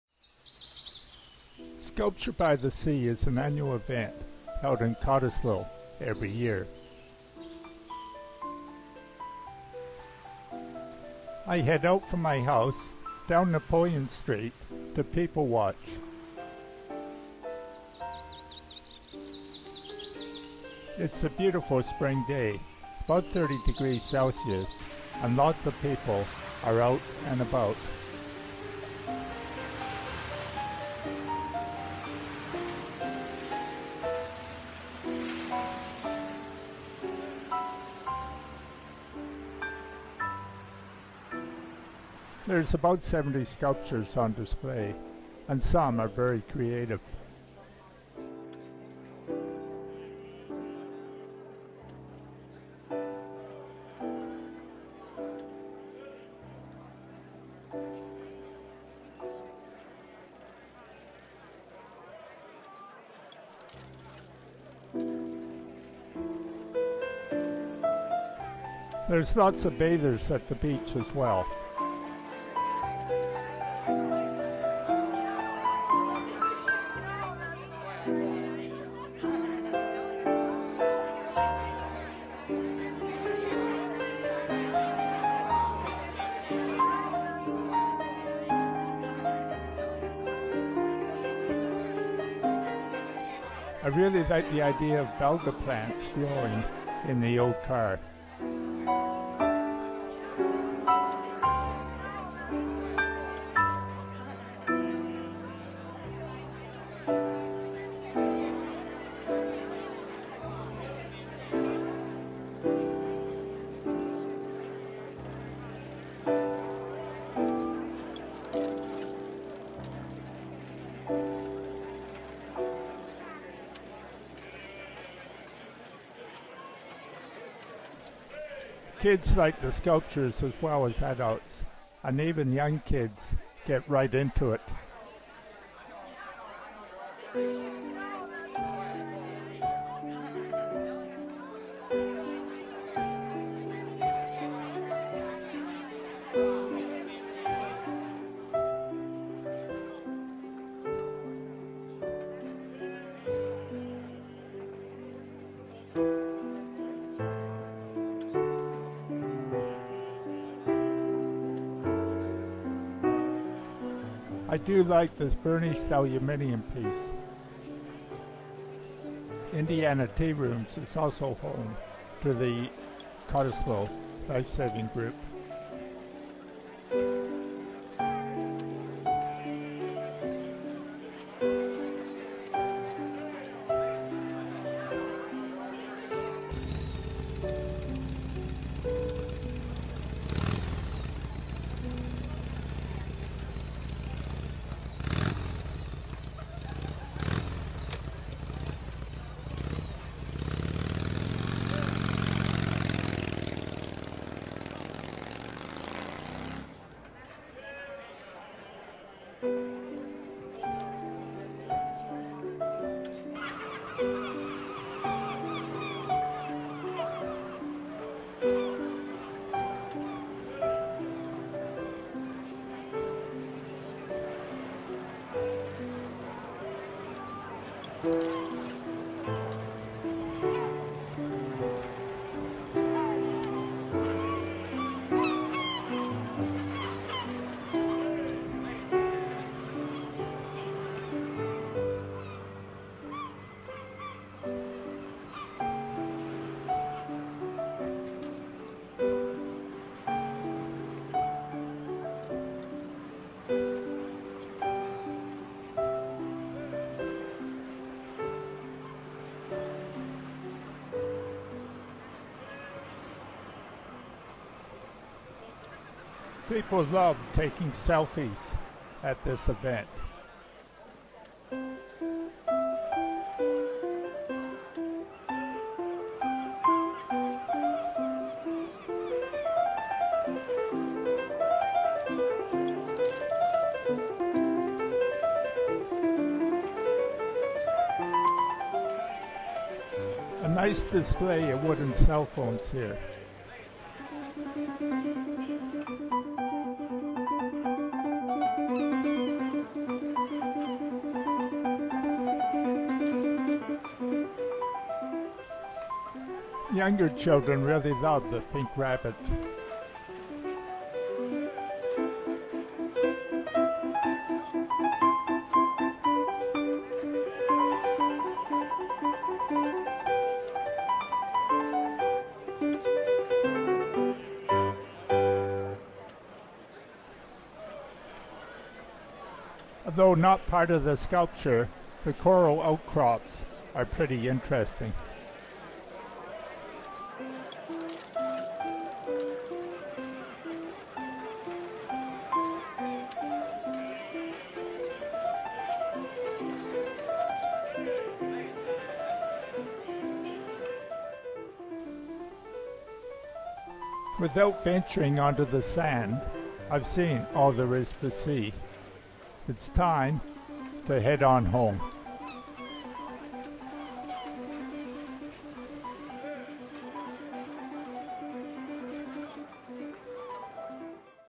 A BUSY DAY AT SCULPTURE BY THE SEA COTTESLOE BEACH 2015 YouTube 1080p
LOCATION
Sculpture by the Sea, Cottesloe is staged on the beautiful Cottesloe Beach, Western Australia. The exhibition is featured from the sea wall all the way along the sand towards North Cottesloe and on the surrounding grassed areas creating a beautiful sculpture park. Access to Cottesloe Beach can be made from the ramps or stairs at different locations along Marine Parade.